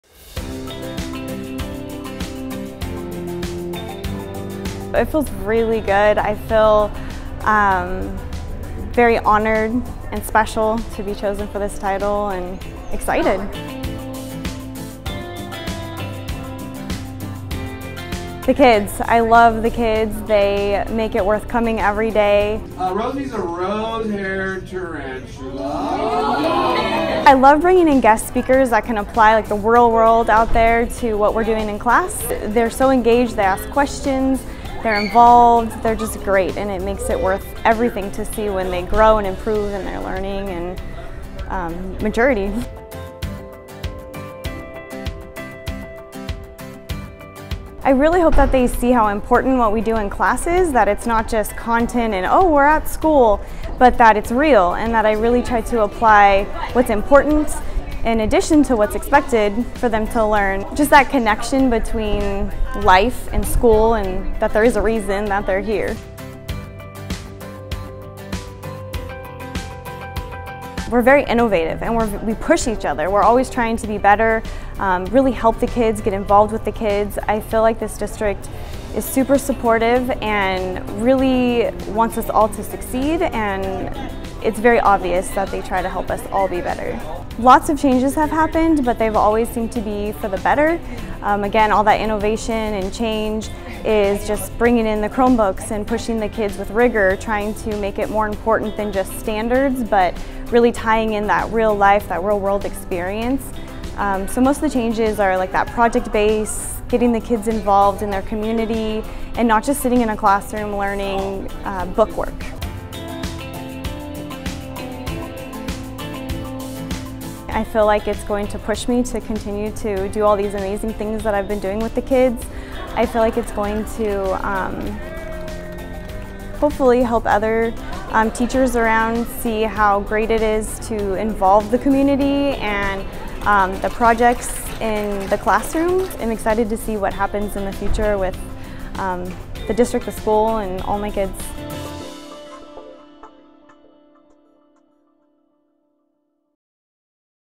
2018-19 Teacher of the Year
Congratulations to Buhach Colony High School Science Teacher Lea Smith on being selected Teacher of the Year for the district! Mrs. Smith loves to provide her students with hands-on experiences to help them connect her lesson plans to the real world